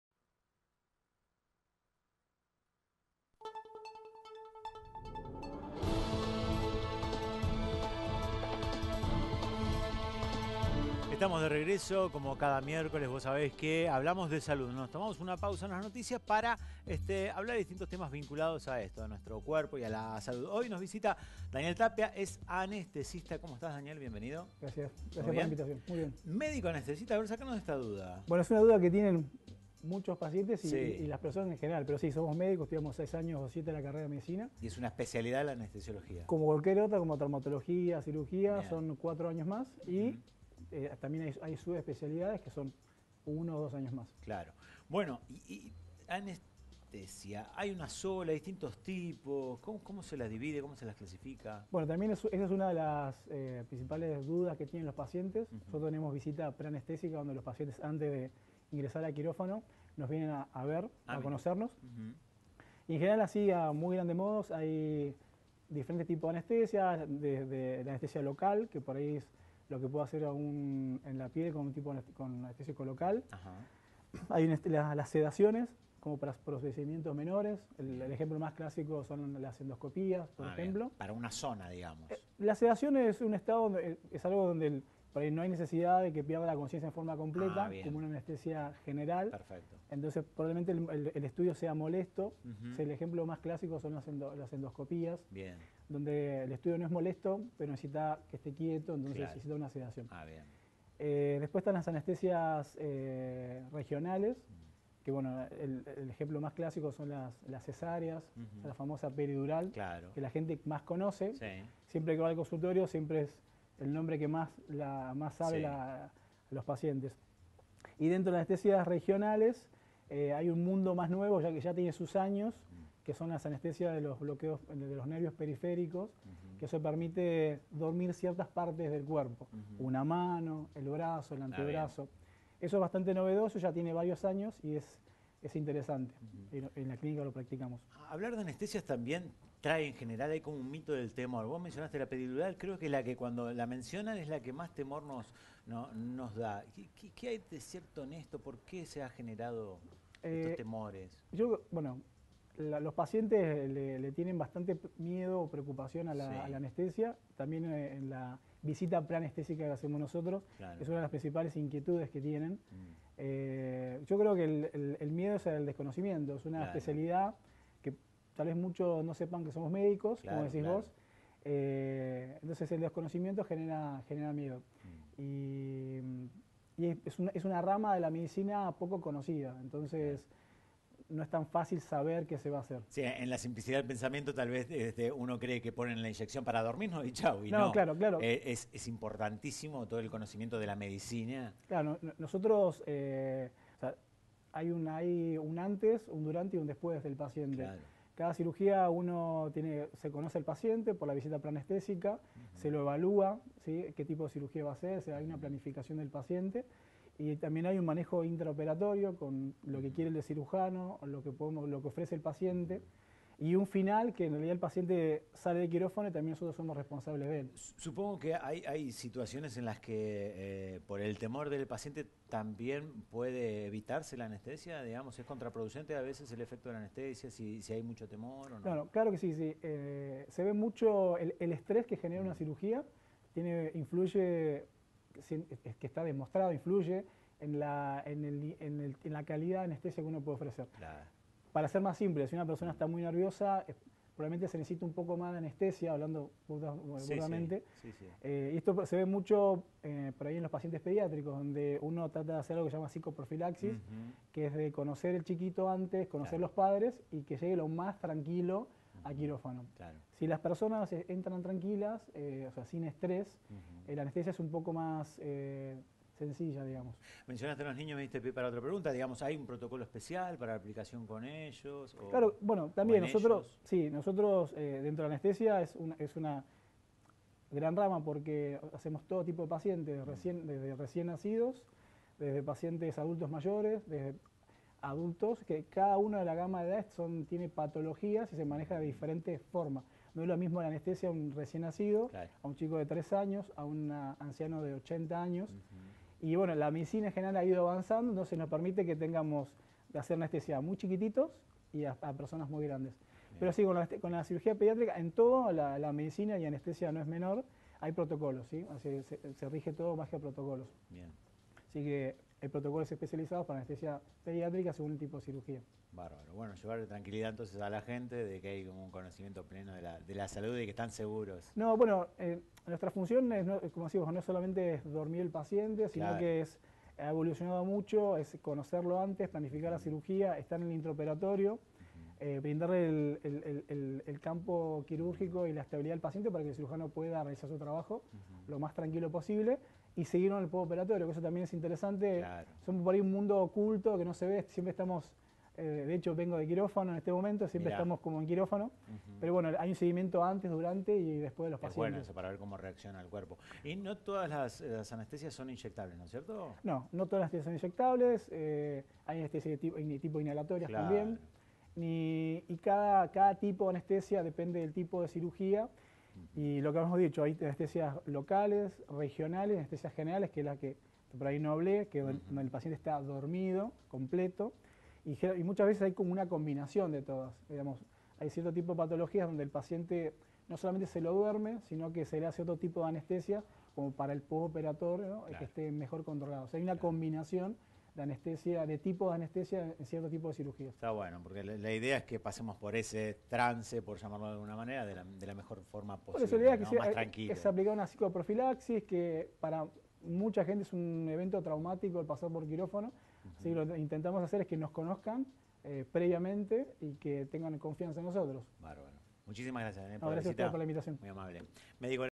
Entrevista | Tipos de Anestesia
En Noticias10 hablamos de Tipos de Anestesia con el Médico Anestesista Daniel Tapia.